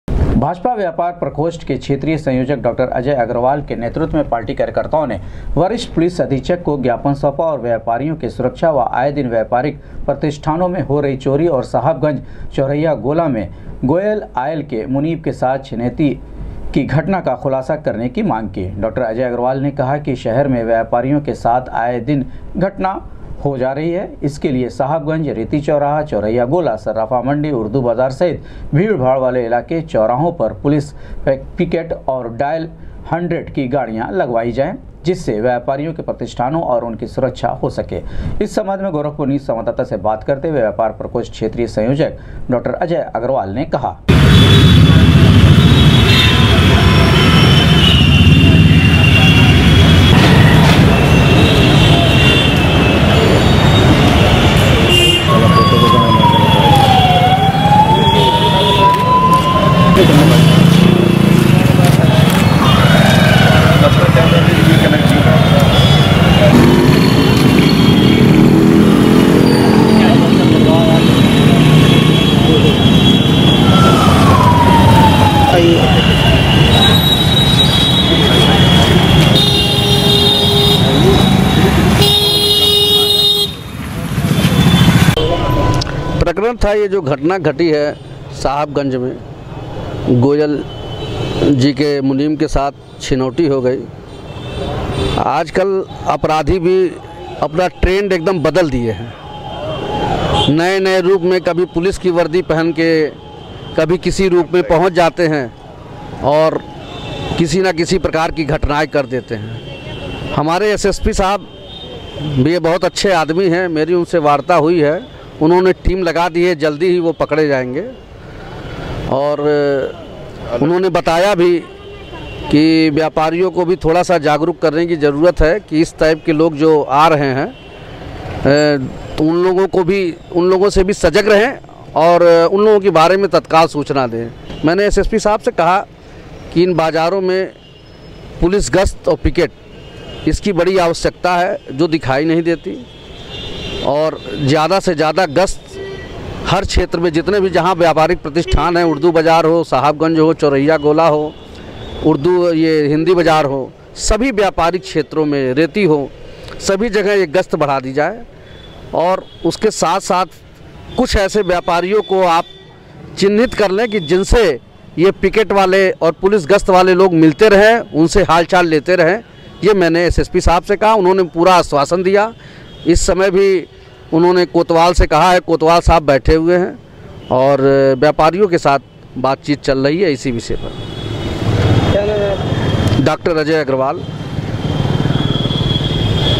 भाजपा व्यापार प्रकोष्ठ के कार्यकर्ताओं ने वरिष्ठ पुलिस अधीक्षक को सौंपा ज्ञापन, सुरक्षा
भाजपा व्यापार प्रकोष्ठ के कार्यकर्ताओं ने वरिष्ठ पुलिस अधीक्षक को सौंपा ज्ञापन, सुरक्षा की कि मांग